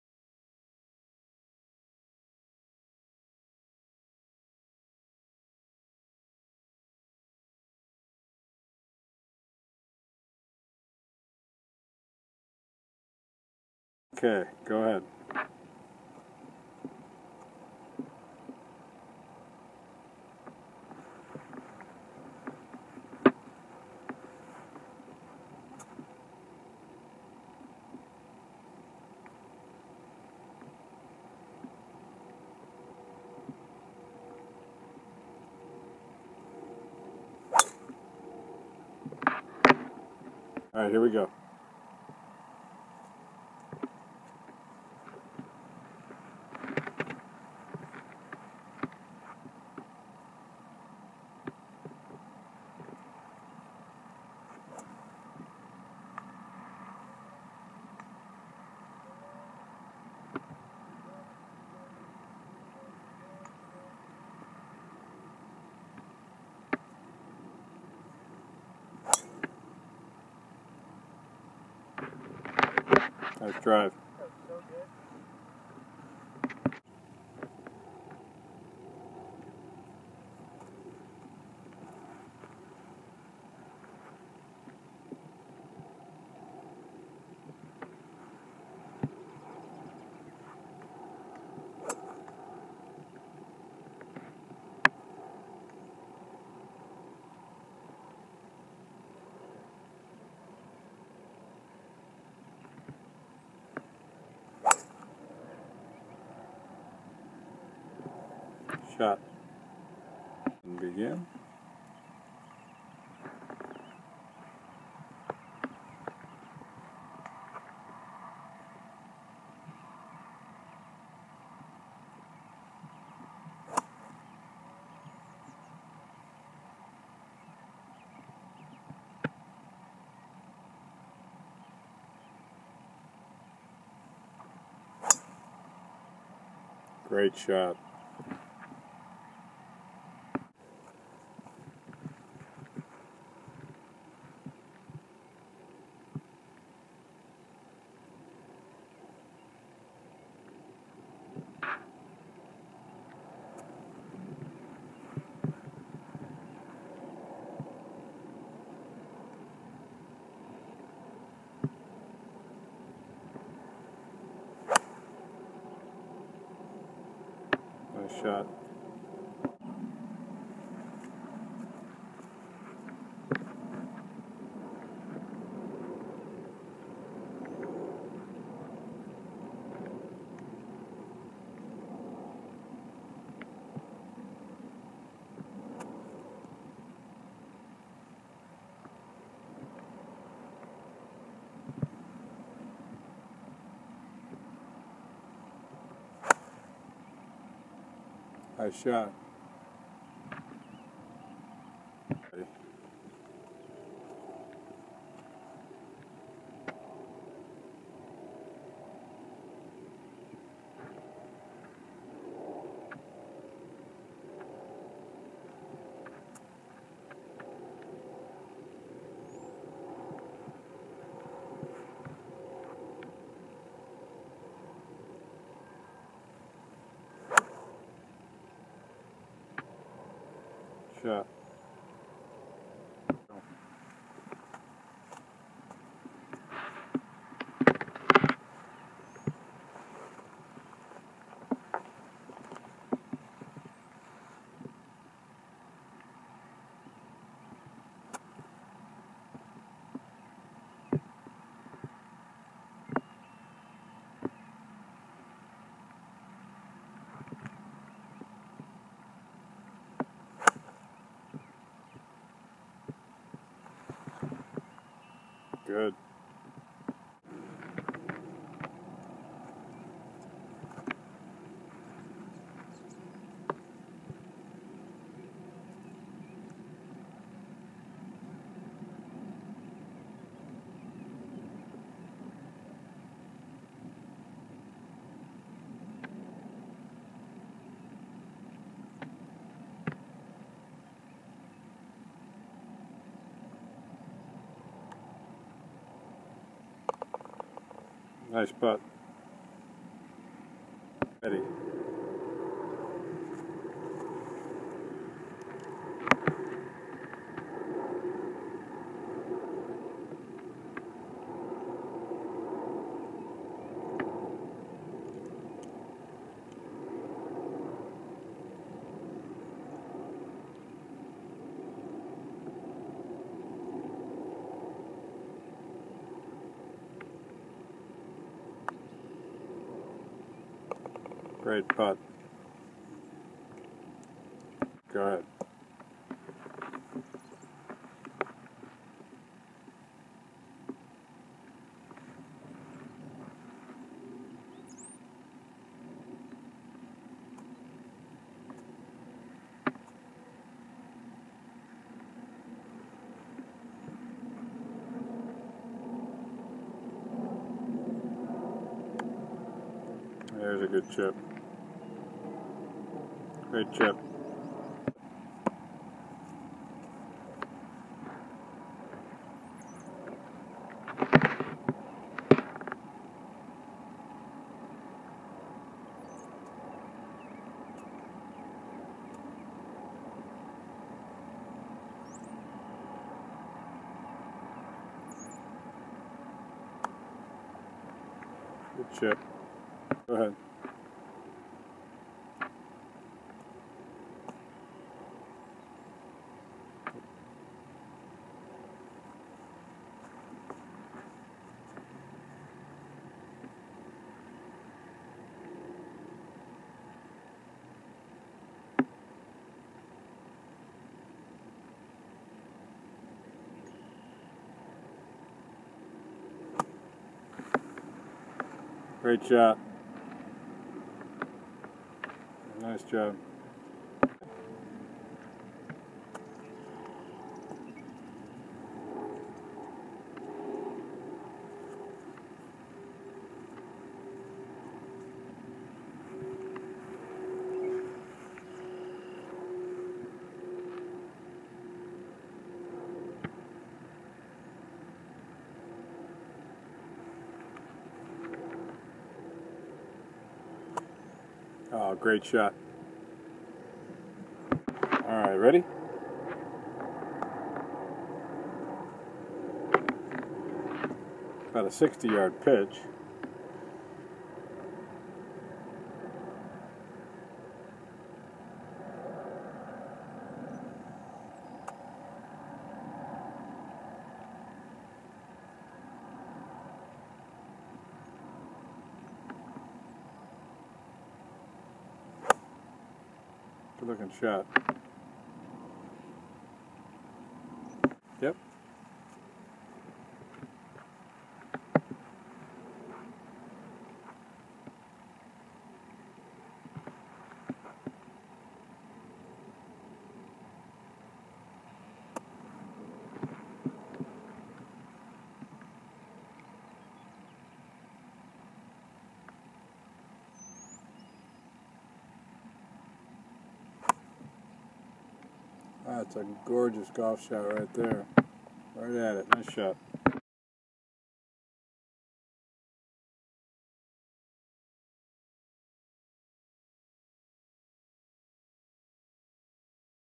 Nasatir, Brandon
Face On & Down the Line clips of Driver, 3 wood, Mid Iron, Wedge, Putt, Chip, Bunker, 60- yard pitch.